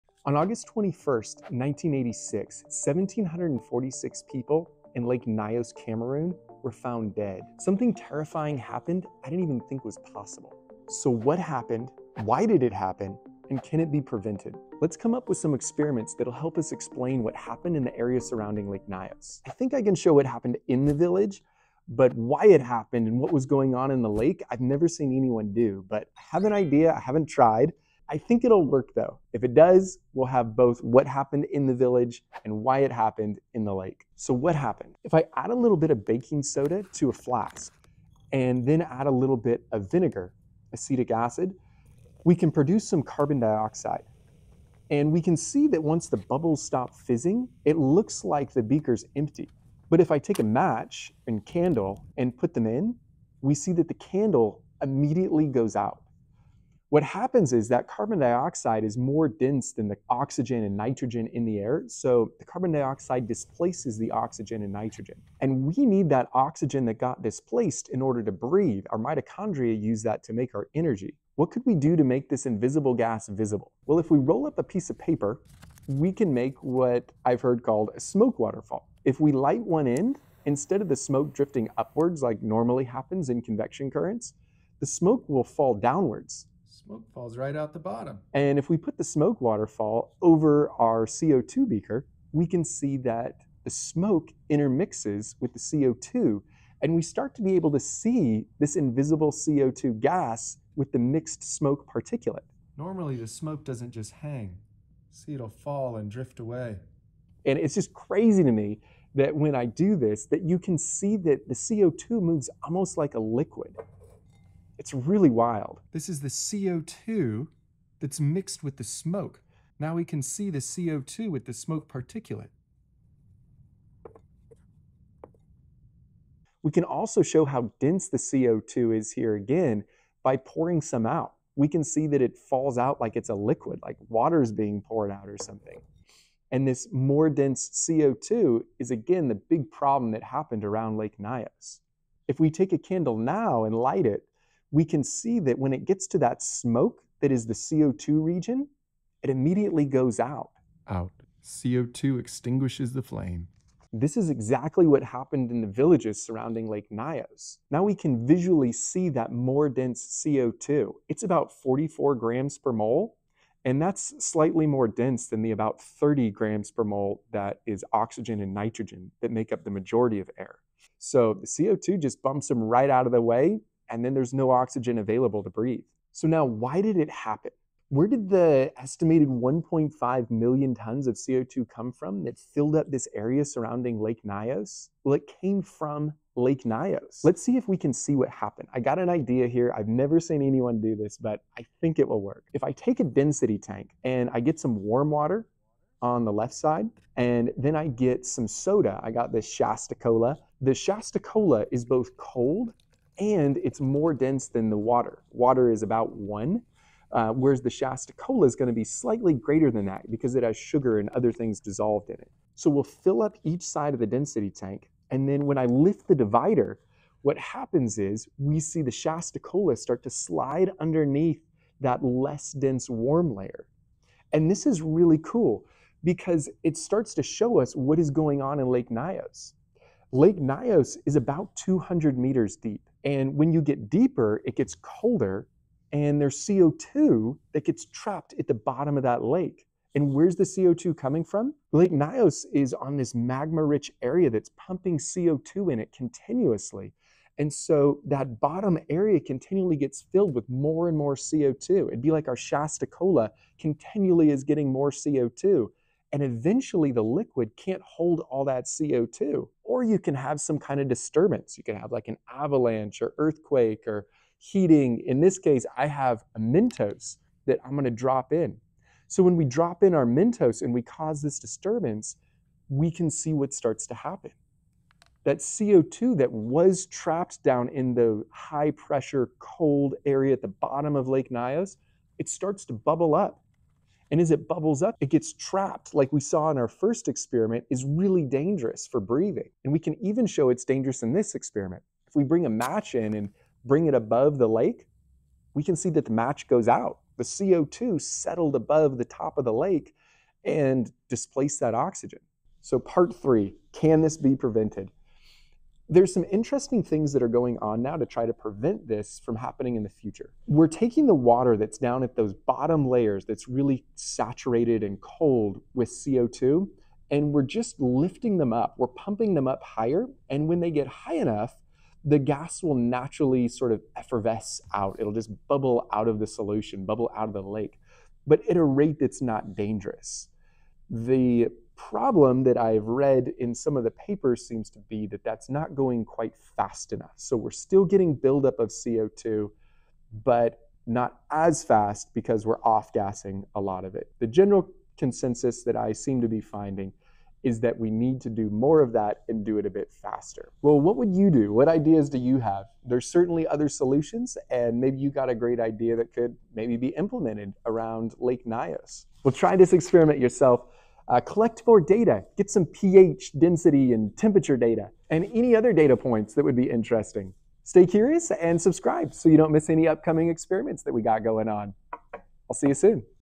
Lake Nyos tragically killed 1,746 villagers.
*A "better explanation of convection currents." I always thought  convection currents happened because "hot things rise..." BUT that is not true! Check it out.
**Let me know what experiments you are curious about in the COMMENTS!

On 21st of August 1986, 1,746 villagers surrounding Lake Nyos were tragically found killed by a mysterious disaster. A limnic eruption occurred that released a mass amount of invisible gas, later discovered to be carbon dioxide. Here is that story with two experiments that help explain what took place. One experiment shows what happened on land and the second experiment examines what took place in the lake and how the CO2 built up. The video concludes by discussing the effort in place to reduce the likelihood of this happening again. There is a great deal more to be said about this event. Many villagers were displaced by the incident and many cattle and animals were killed. I would encourage you to search up the event. Read some papers. If you get the chance, report back any interesting things that you learn.

Stay curious!
-Flemdog